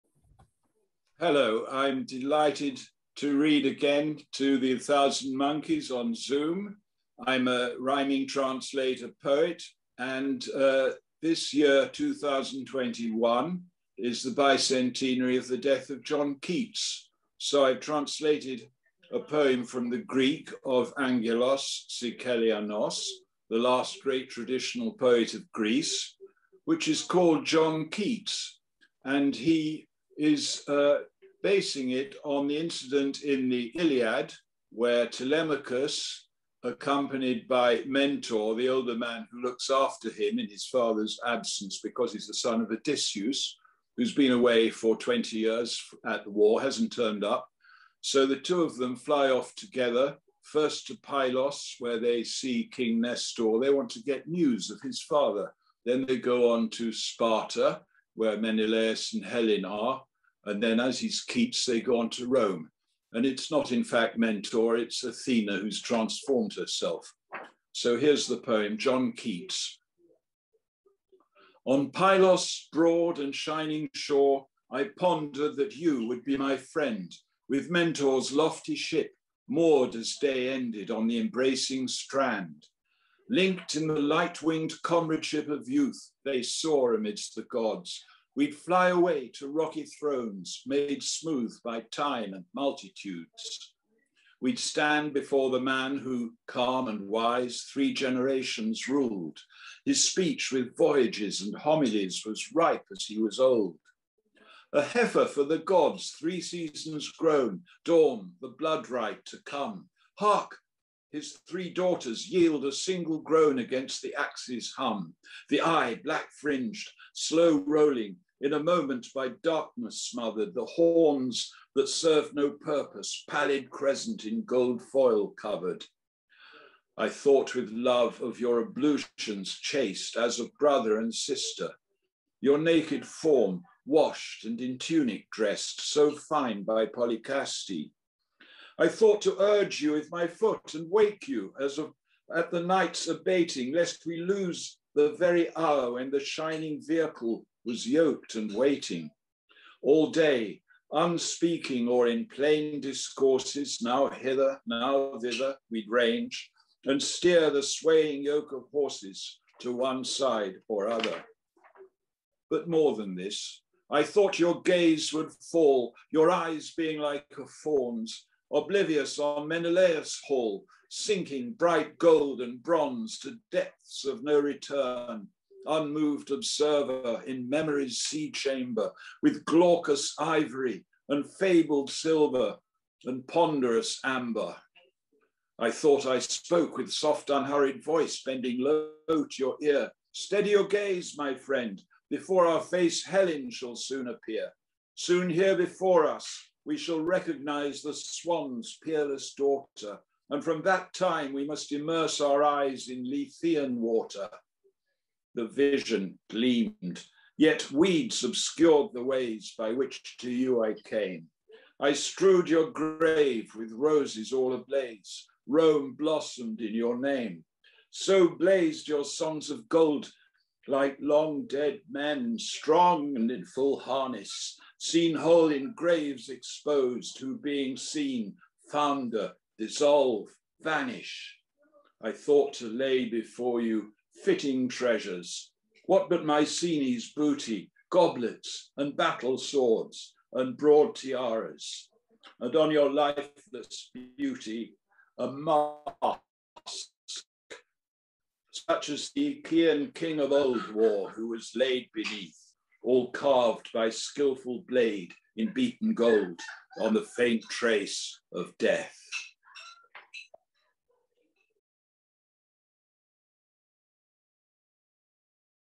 Timothy Adès reads his translation of 'John Keats' by Ángelos Sikelianós
Timothy Adès reads Angelos Sikelianós poem, to commemorate the bicentenary of Keats' death.